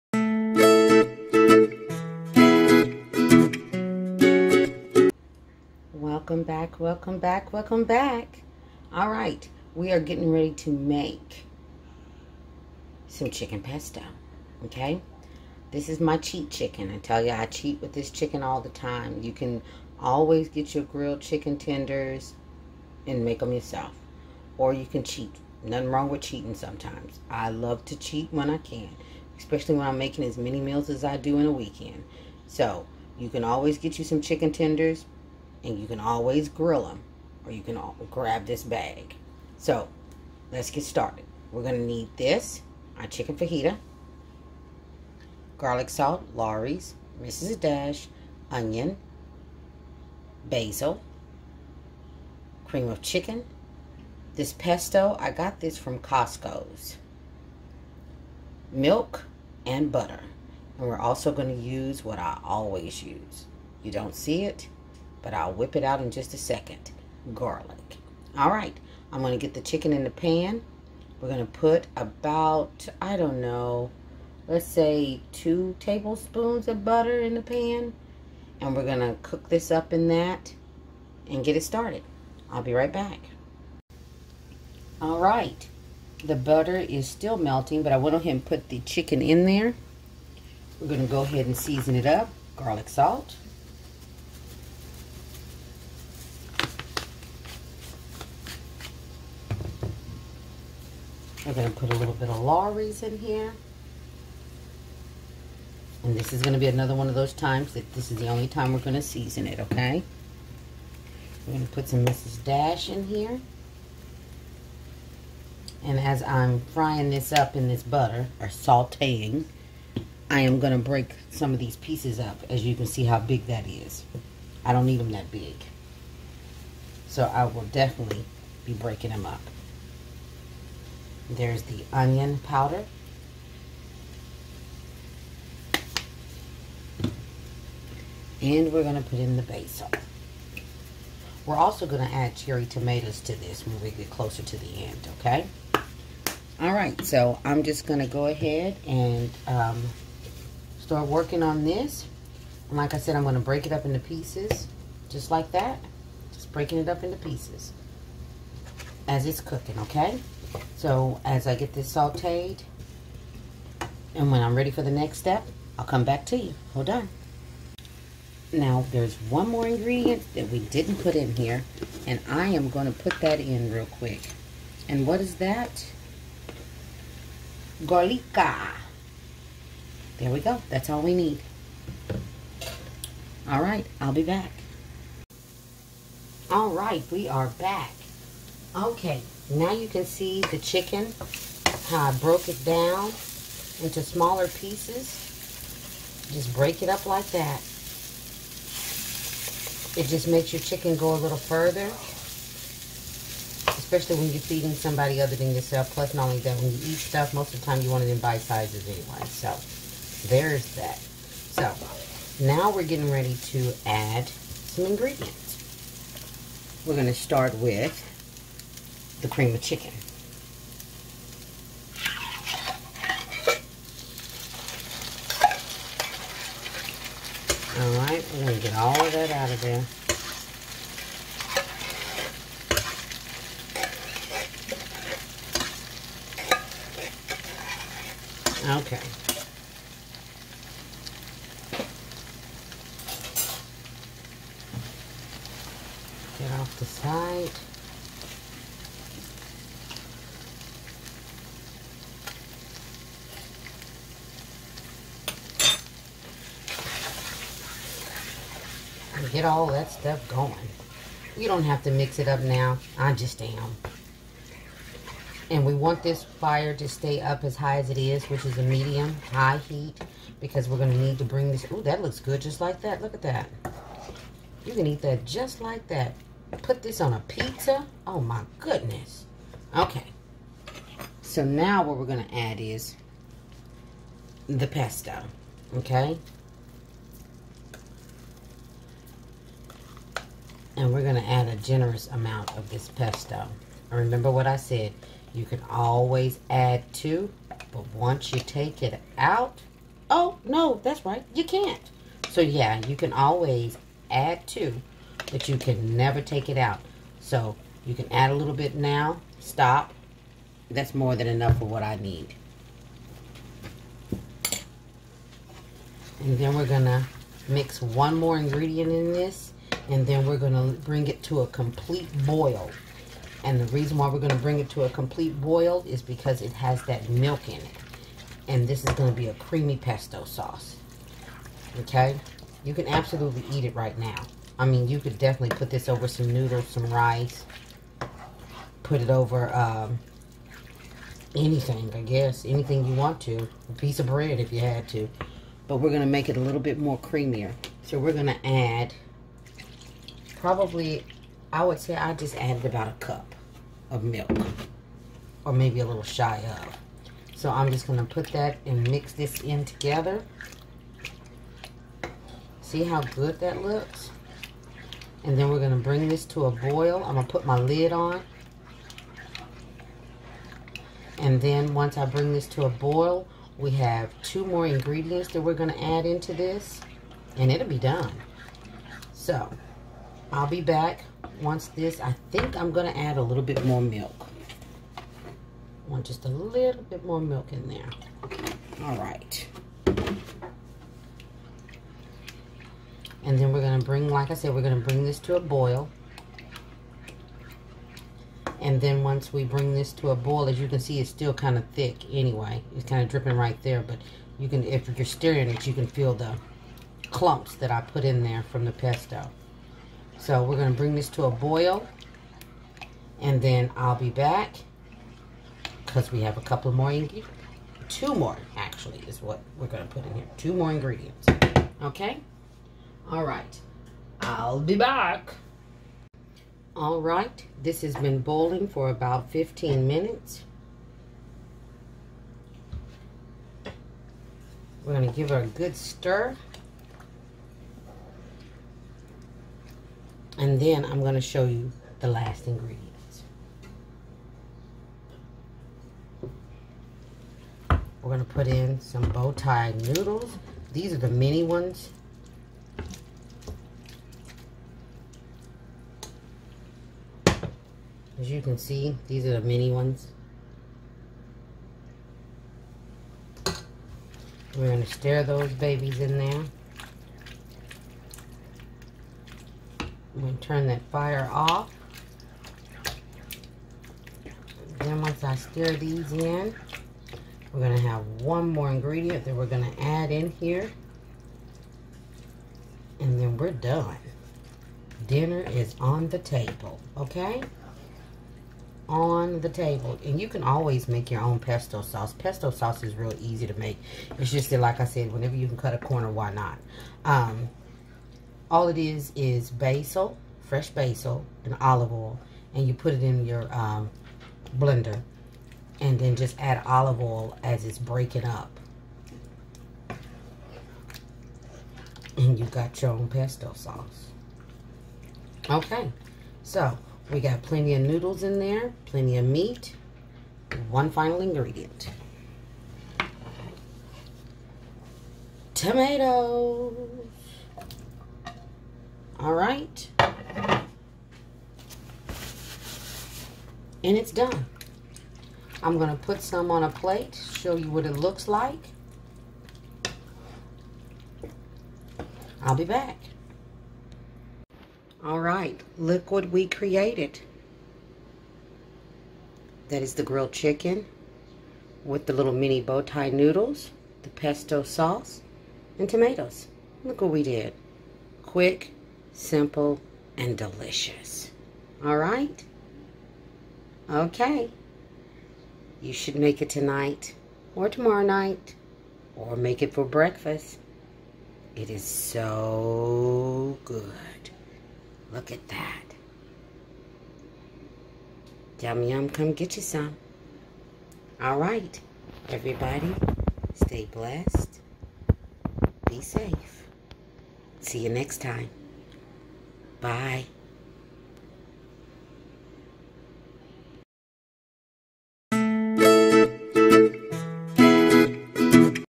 How to Make Creamy Pesto Chicken
This is a Creamy Chicken Pesto Pasta, it's loaded with juicy chicken and tomatoes.  Enjoy!

If I can do it, you can do it.  Now let's get cooking.